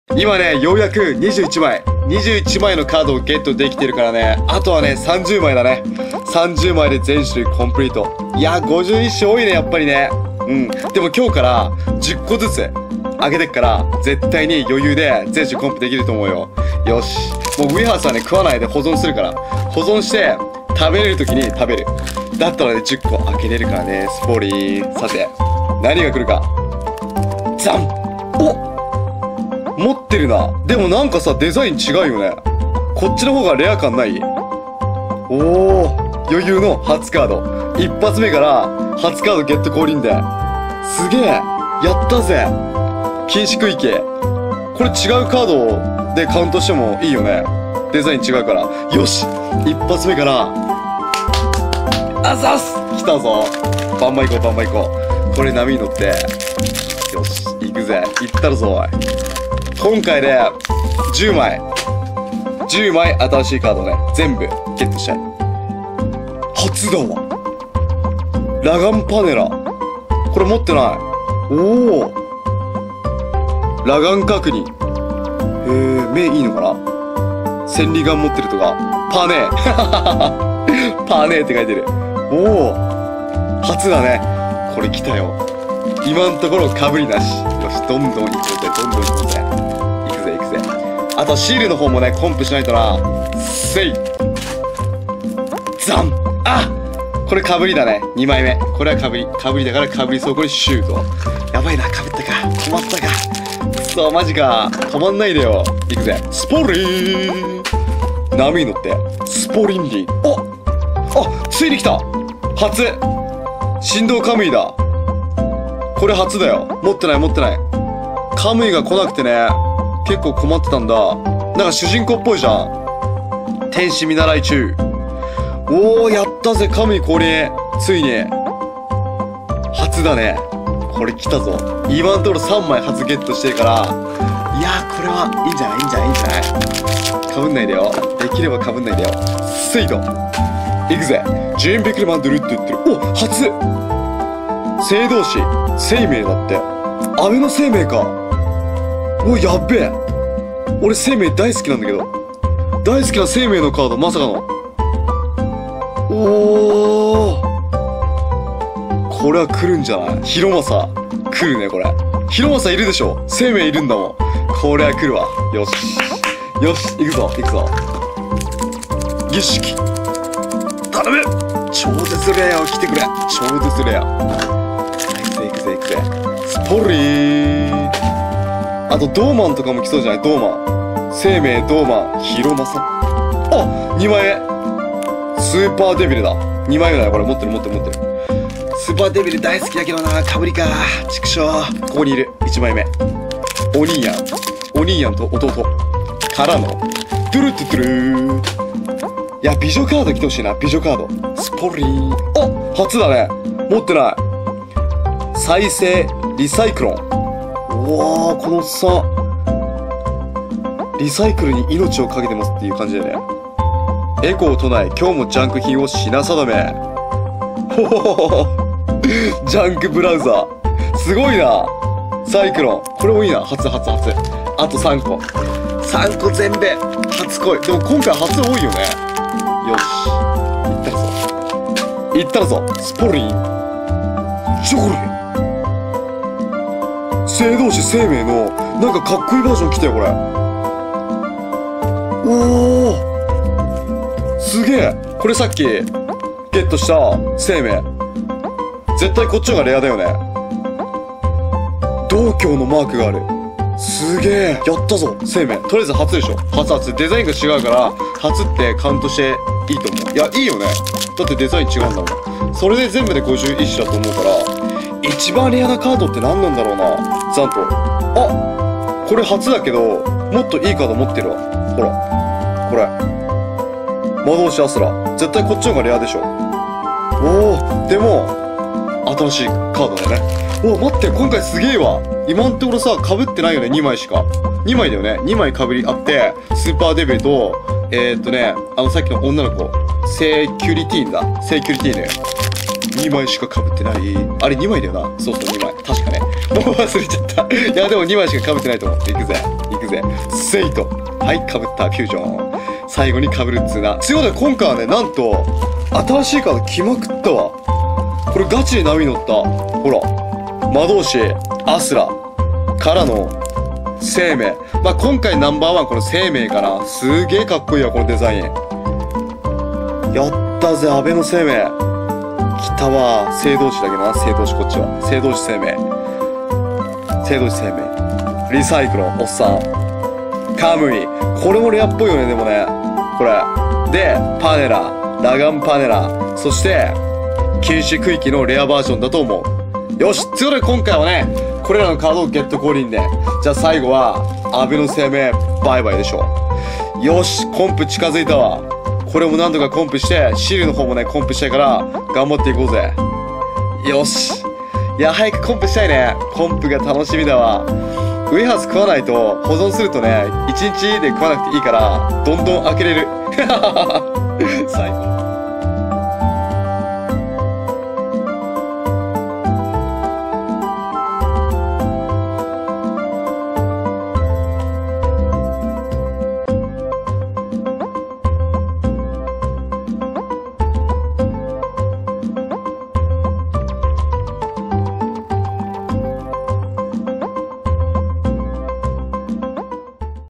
神ビックリマンチョコ（10個開封！） 神威転生 「全種類コンプリートまであと30枚！」 PART 5 食玩レビュー！
今回は初カードがたくさん来ました(*´ω｀*)
セイメイ超かっこいい！！